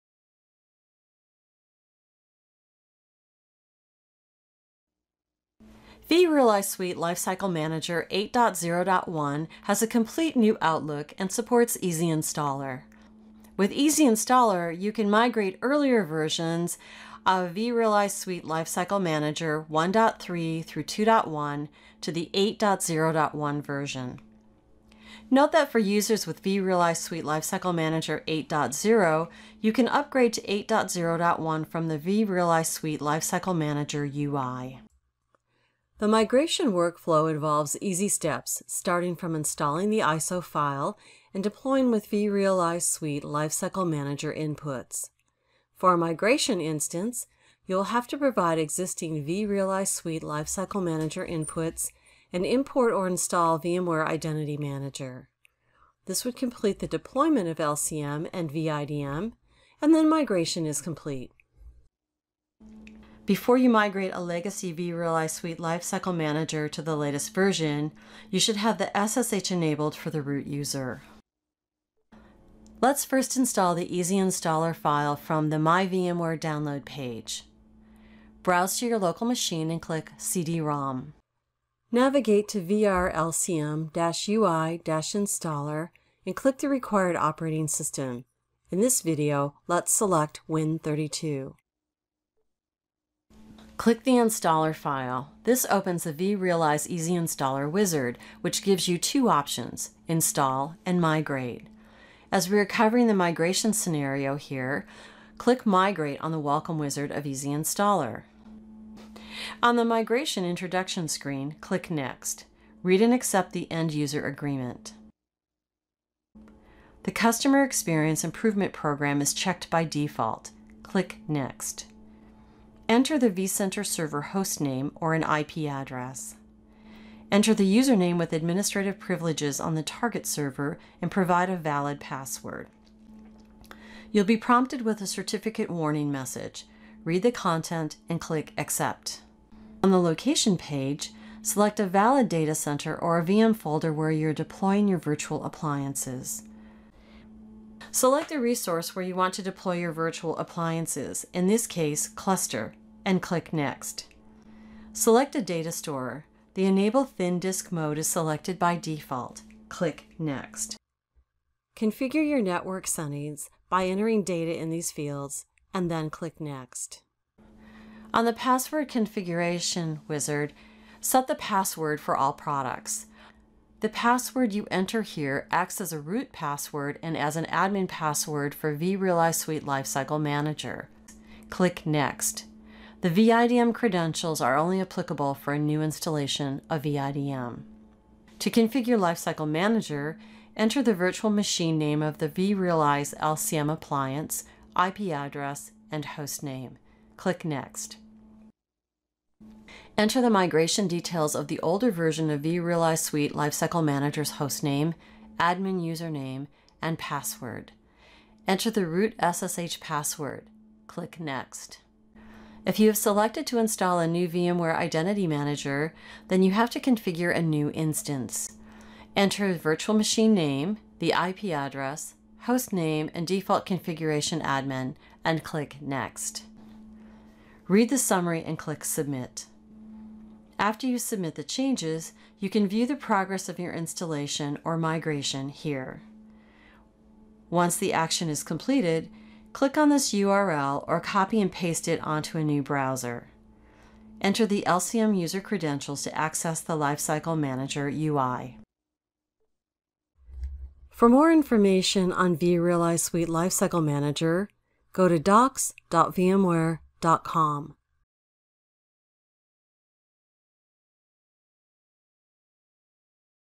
Migrating to vRealize Suite Lifecycle Manager 8.0.1 Using the vRealize Easy Installer
The vRealize Suite Lifecycle Manager 8.0.1 version has a complete new outlook and supports Easy Installer. With Easy Installer, you can migrate from the earlier versions of vRealize Suite Lifecycle Manager to the 8.0.1 version or later. To learn more about the Easy Installer, see vRealize Suite Lifecycle Manager documentation.